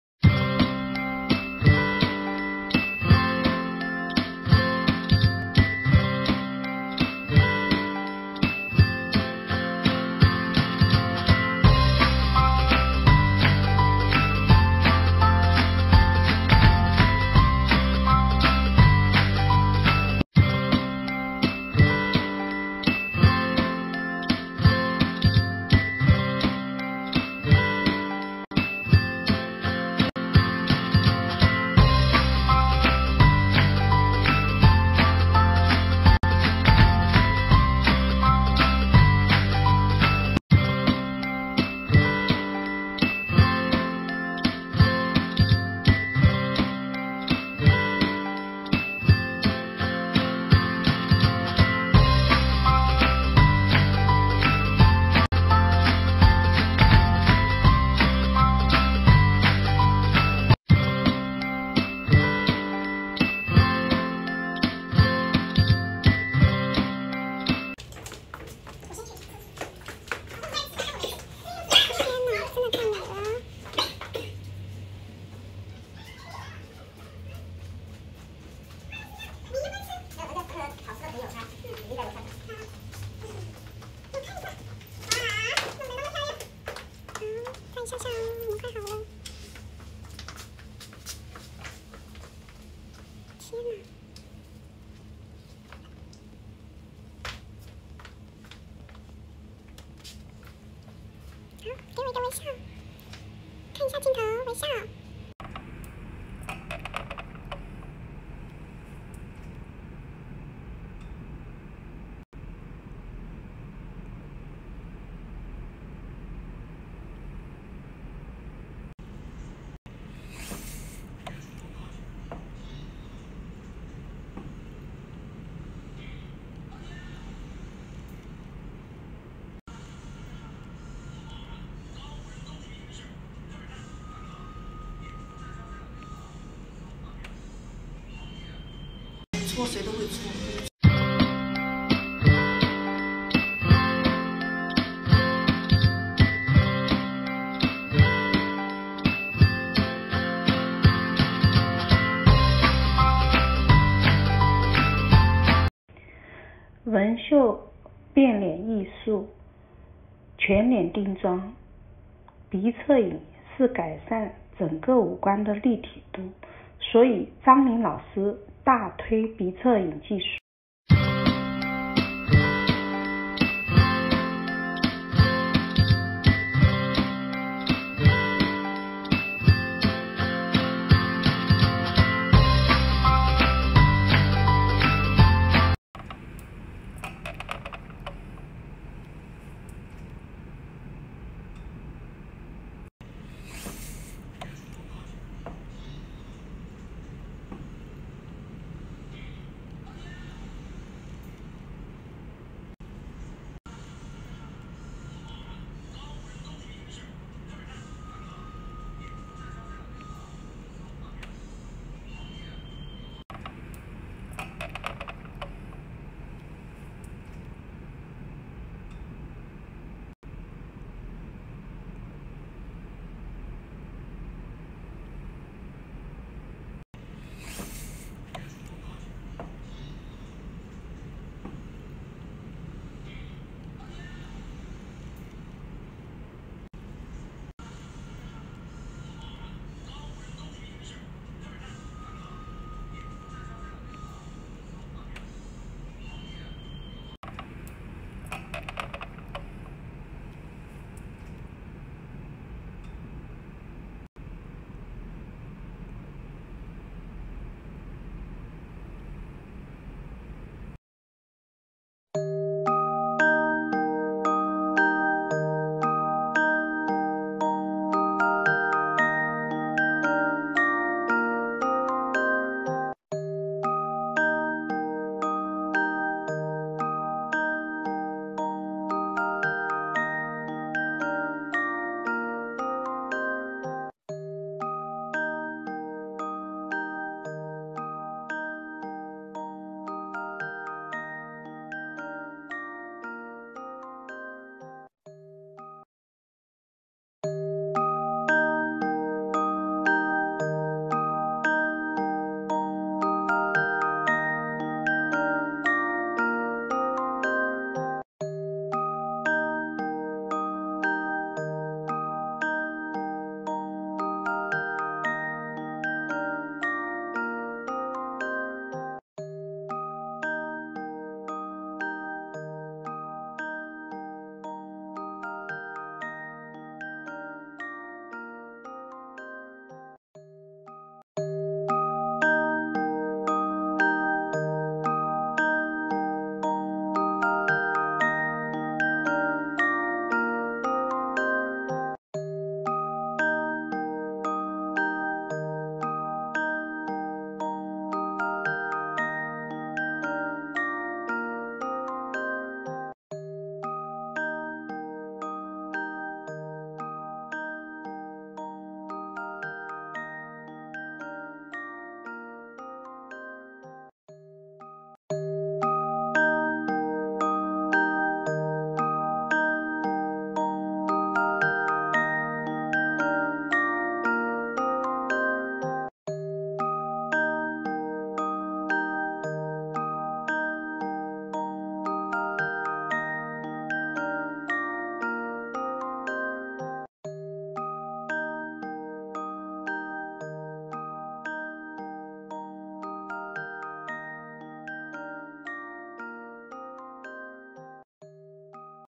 ☺︎ 看她的眉毛↓ 高傲的眉峰，細而舒的眉尾，這樣才高級 既能突出眉骨的立體感又有自然柔和的小靈氣， 鼻側影淡淡浮上鼻兩側、美爆了有木有！台灣張靈紋繡教學作品
台灣張靈紋繡教學作品